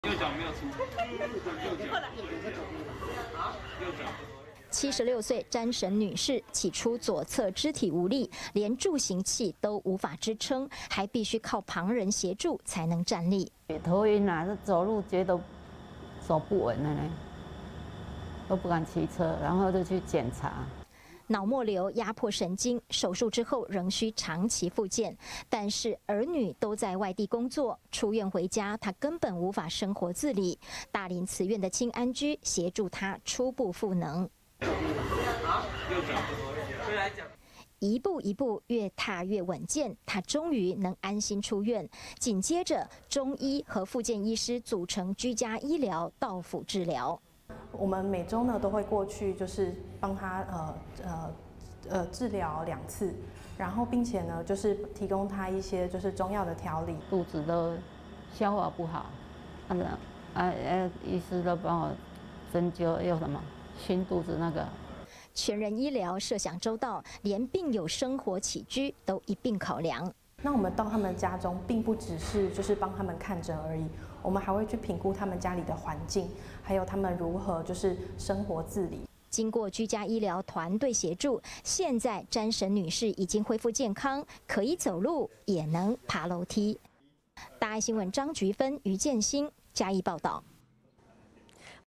大林慈院居家醫療 中醫+復健一站式照護
讓治療有更好效果，再來這位患者是詹沈女士，腦膜瘤開刀後，大林慈濟醫院、啟動居家醫療，中醫和復健科、接續到府協助復原。

76歲詹沈女士起初左側肢體無力，連助行器都無法支撐，還必須靠旁人協助才能站立。

病友 詹沈女士：「頭暈 走路覺得走不穩，不敢騎車 就去檢查。」

腦膜瘤壓迫神經，手術之後仍需長期復健，但是兒女都在外工作，出院回院根本無法生活自理，大林慈院輕安居協助她初步復能。

一步一步越踏越穩健，她終於能安心出院，緊接著中醫和復建醫師組成居家醫療到府治療。

大林慈院中醫師 鍾郁蓉：「我們每周都會過去幫她治療兩次，並且提供一些中藥調理。」

病友 詹沈女士：「肚子都消化不好，醫師都幫我針灸 清肚子。」

全人醫療設想周到，連病友生活的起居都一併考量。

大林慈院中醫師 鍾郁蓉：「我們到他們家中，並不只是幫他們看診而已，我們還會評估他們家裡的環境，還有他們如何生活自理。」

經過居家醫療團隊協助，現在詹沈女士已經恢復健康，可以走路，也能爬樓梯。

張菊芬 于劍興  嘉義報導